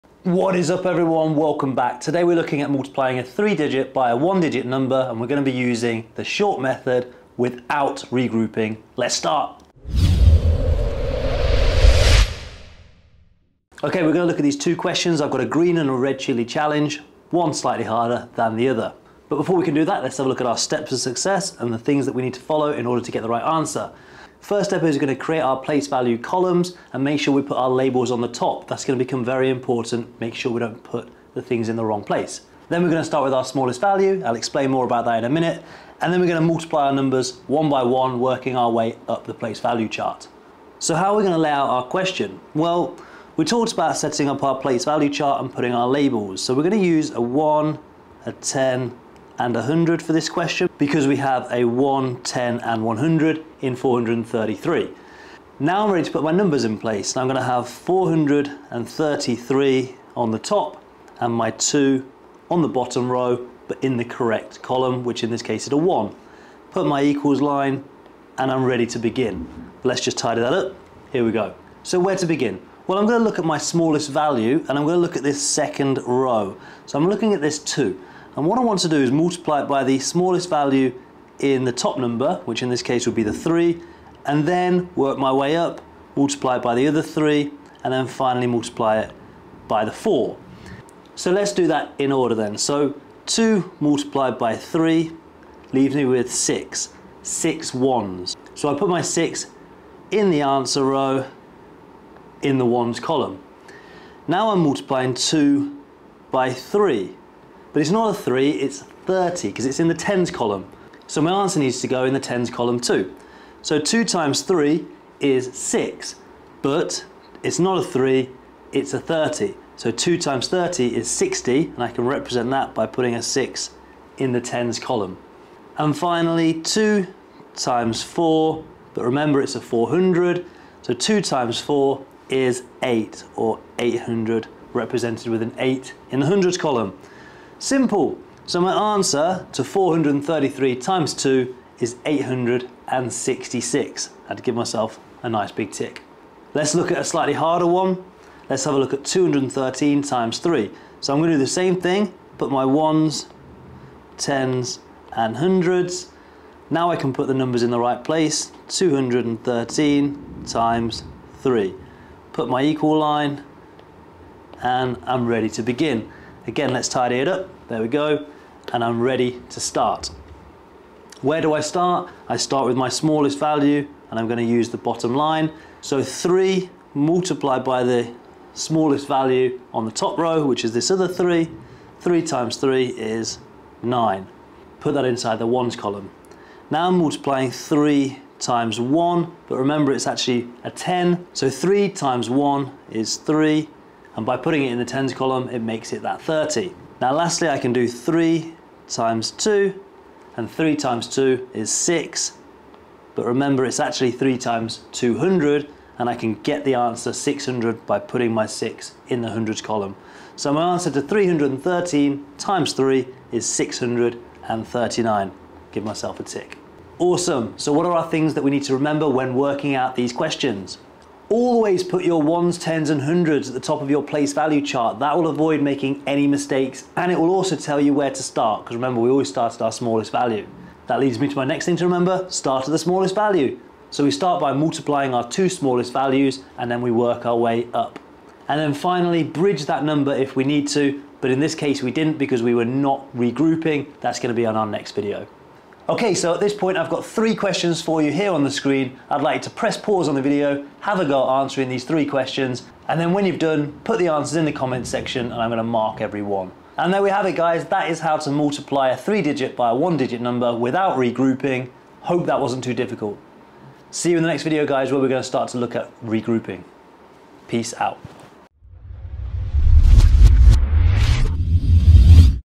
Multiplication 3 Digit By 1 Digit Without Regrouping | The Maths Hero
In this maths lesson we will be learning how to solve multiplication 3 digit by 1 digit questions without regrouping. We will be using short, column method to solve these questions. We will begin by looking at our steps to success for this problem. This begins by setting up our question correctly into column method, then we will begin solving the question by answering the smallest value first, finally we will work our way up the place value chart,

Hope this video helps, enjoy!